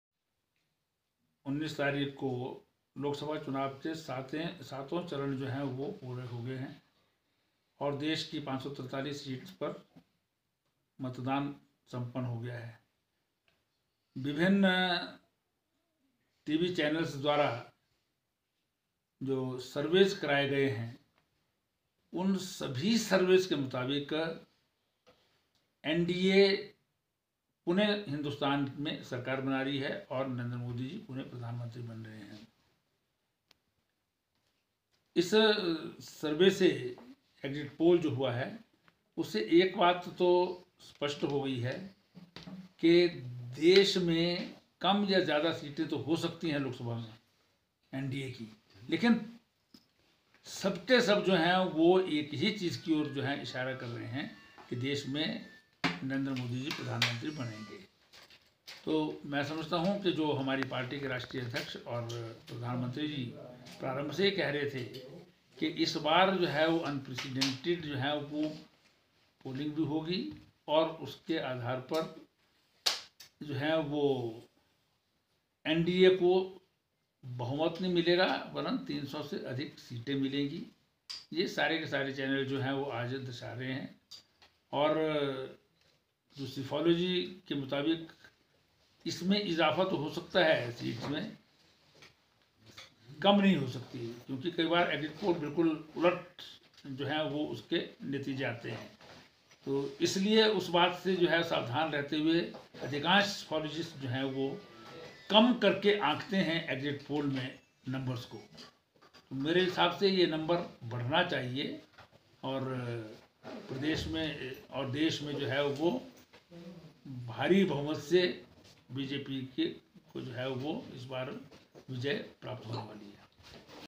शिक्षा मंत्री सुरेश भारद्वाज बोले....सम्पन्न हुए चुनावों में सबका इशारा एक ही तरफ...कम ज्यादा हो सकत
शिक्षा मंत्री सुरेश भारद्वाज बोले. सम्पन्न हुए चुनावों में सबका इशारा एक ही तरफ...कम ज्यादा हो सकती हैं सीटें. भाजपा बना रही केंद्र में सरकार